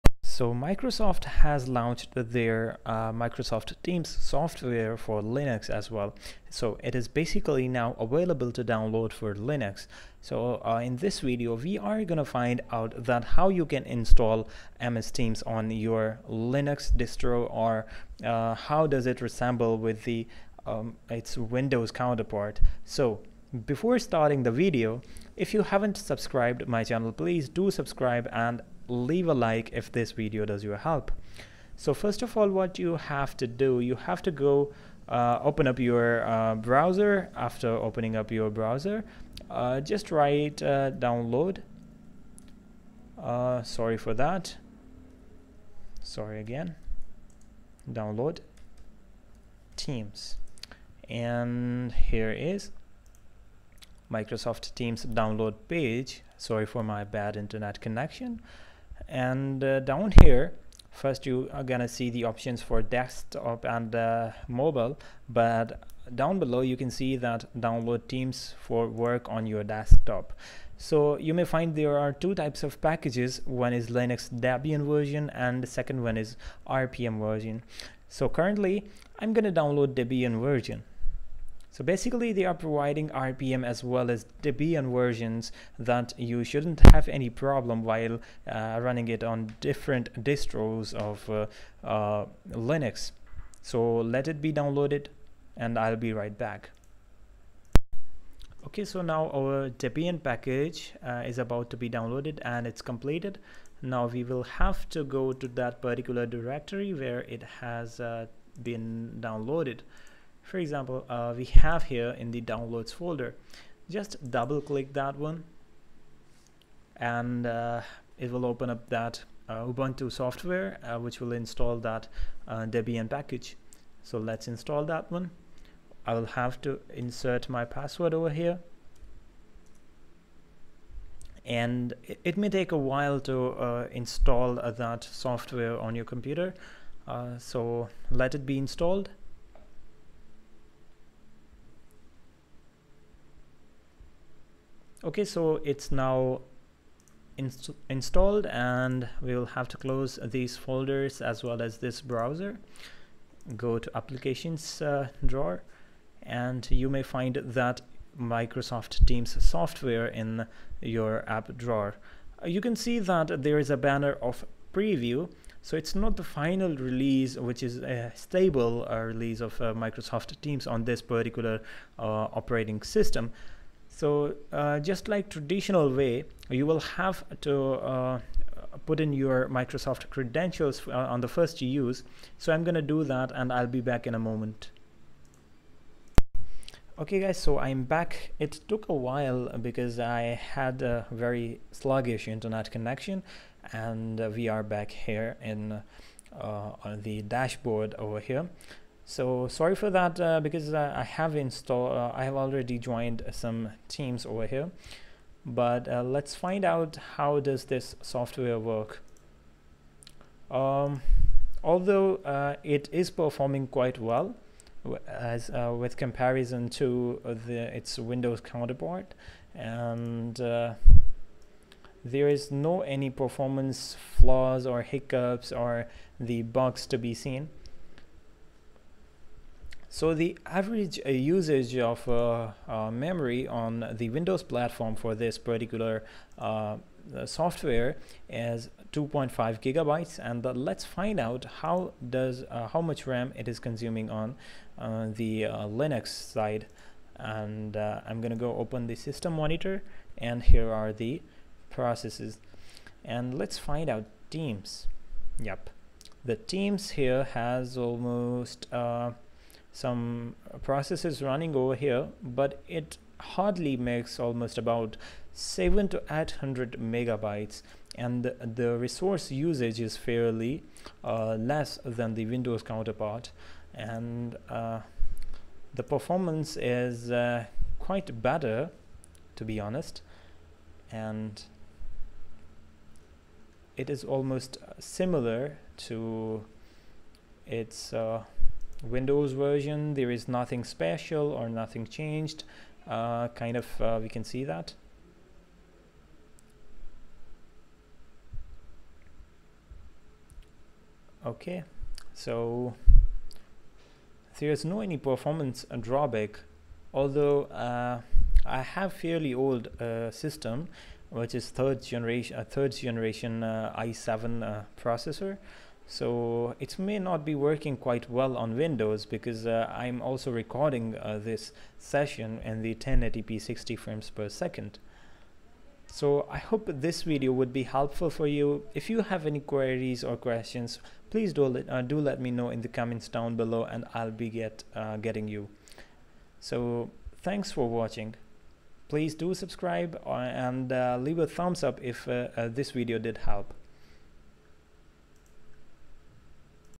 Microsoft Teams for Linux || Installation & Review
Microsoft Teams is Now Available on Linux. Microsoft Teams is a team communication service similar to Slack. While Slack offers a native Linux client, a lot of users were waiting for Microsoft Teams to be available on Linux. And, now, finally, Microsoft Teams has arrived for the Linux platform.

For any suggestion or complaints, please comment.